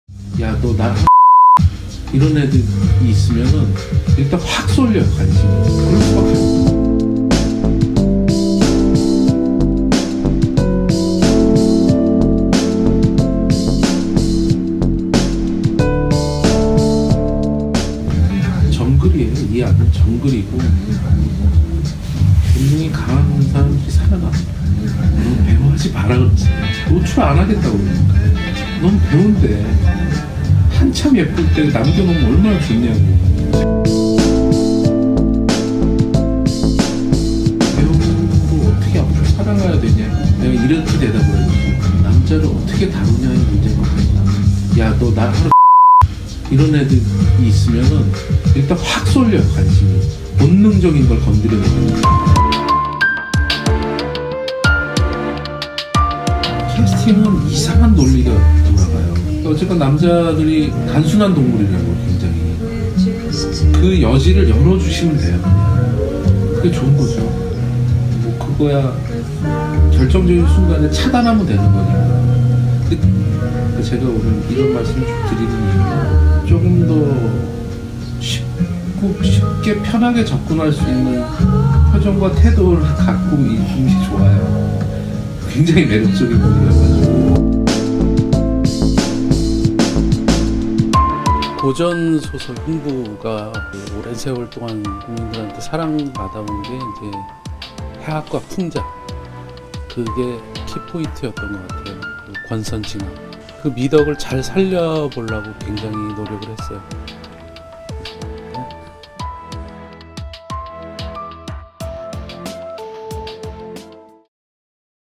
감독이 오디션 보러 온 여배우에게 한 말
"영화 '흥부'에 권선징악을 잘 살려보려고 노력했어요"